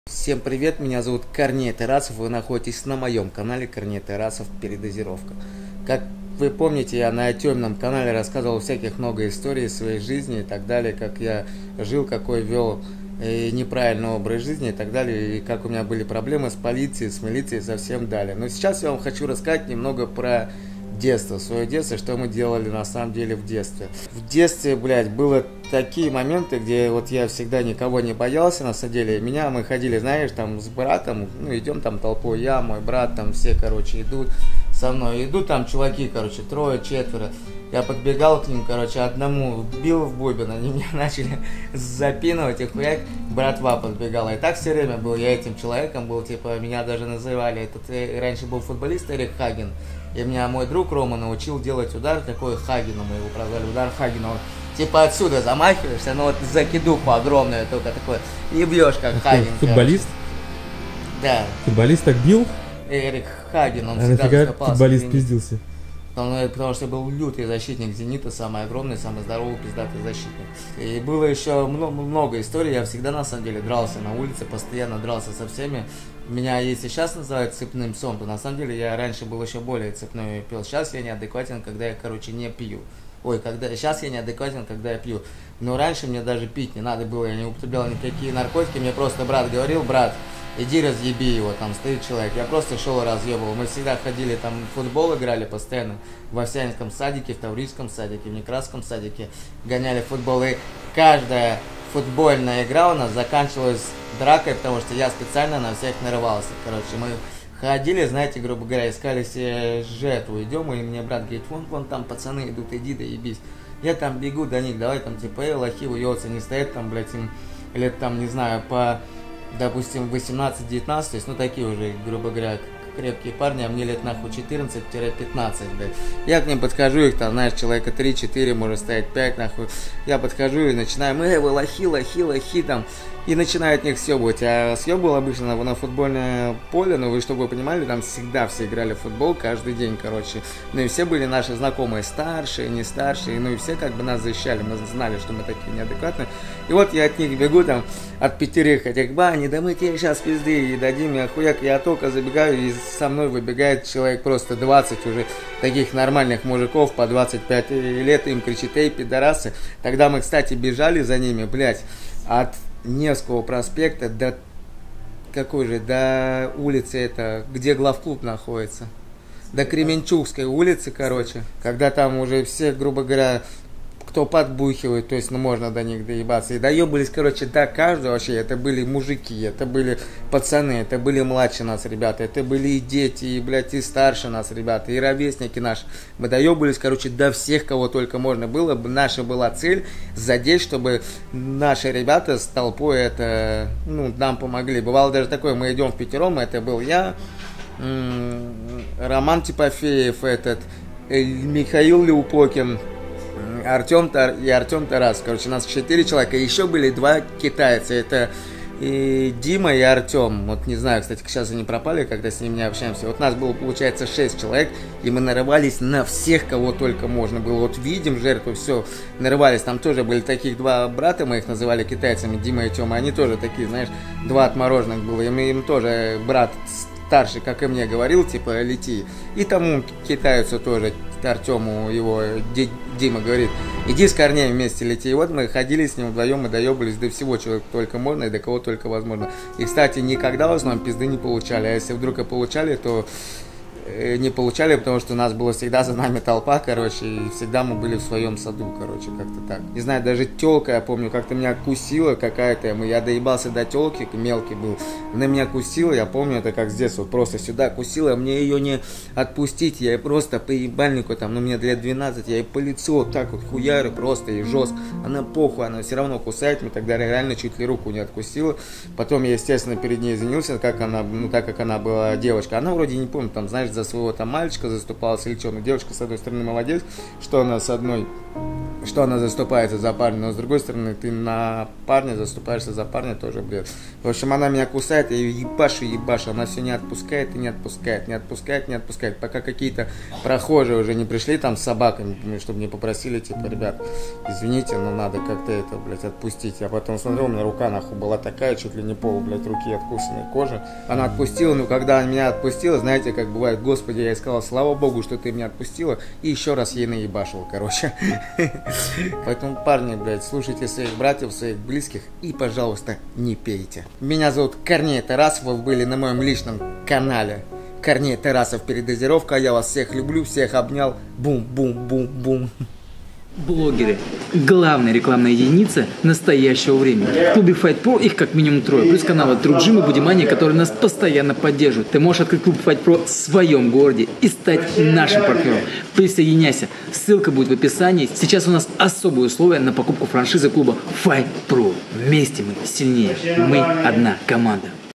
ДЕВУШКА ЧУТЬ ЛИ НЕ ОТКУСИЛА КОРНЕЮ ТАРАСОВУ РУКУ В УЛИЧНОЙ ДРАКЕ
История из жизни Корнея Тарасова.